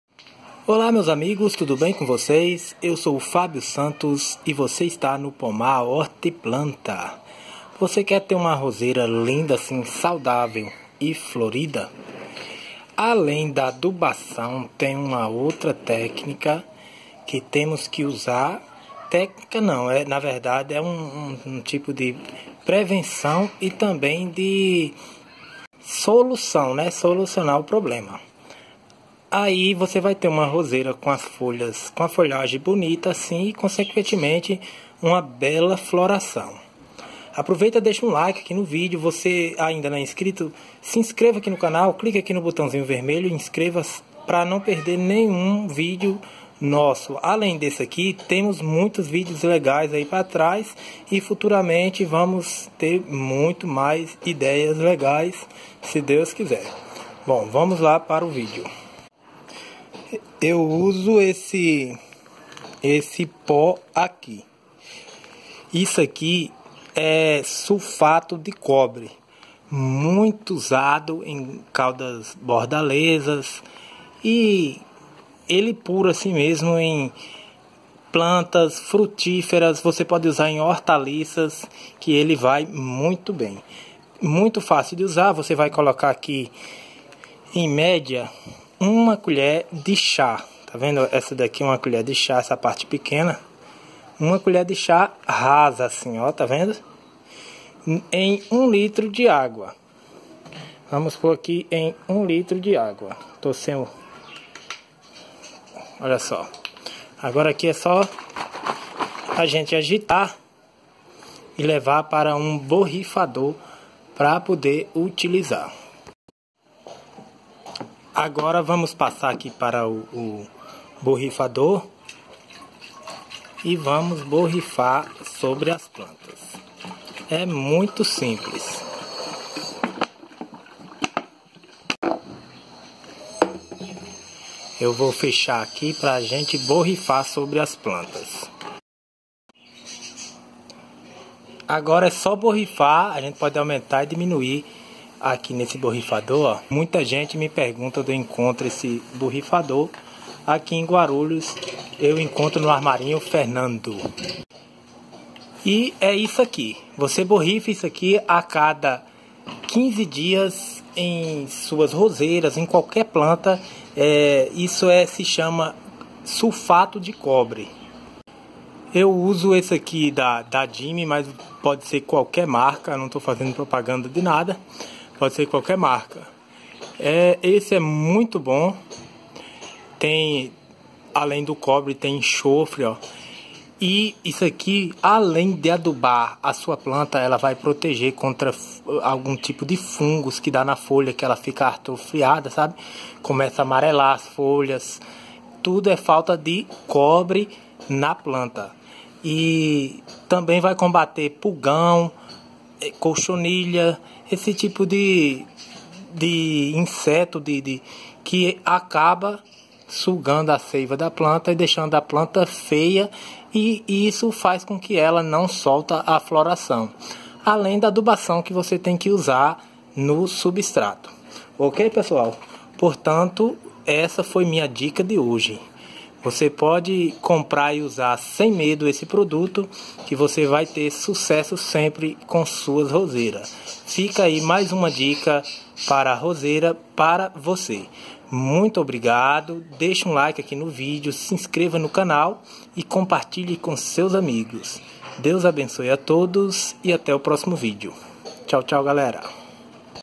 Salve sua ROSEIRA com esse líquido!
Esse líquido cura sua roseira, deixa forte e linda

Nesse vídeo apresento um adubo mineral que é essencial para ter uma roseira saudável e muito linda.

Conheça meu canal de vendas Orquidea's Continental

Muito obrigado

Coloquei esse líquido em minhas ROSAS e veja que incrível